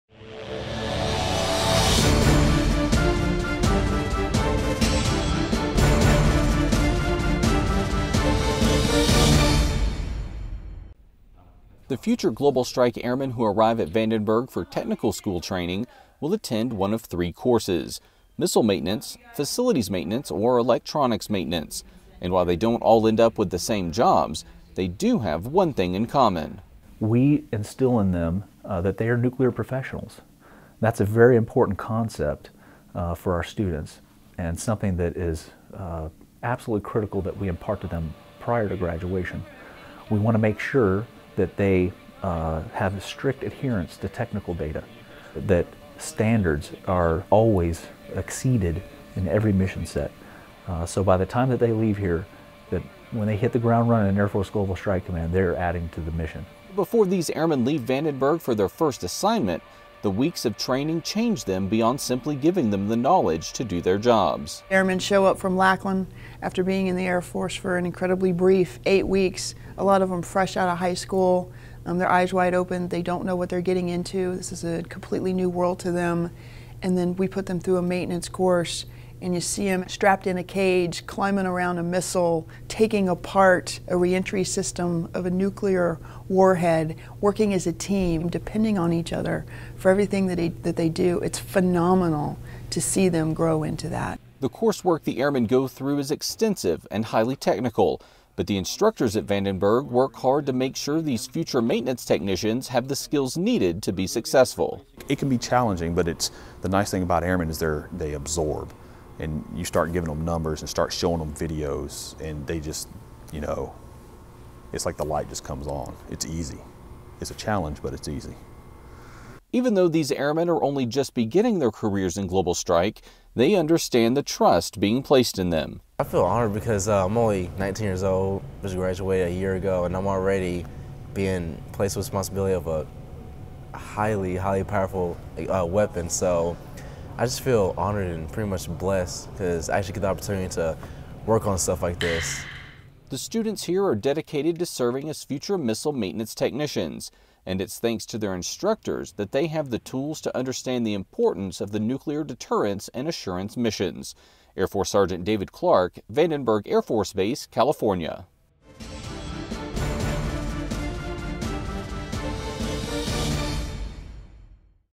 532nd Training Squadron initial missile maintenance training
Vandenberg Air Force Base, Calif., plays an important role in the life of every new Air-Launched Cruise Missile and Intercontinental Ballistic Missile maintenance Airman. Staff Sgt. David Clark introduces us to the Airmen responsible for training the future missile maintainers of Air Force Global Strike Command.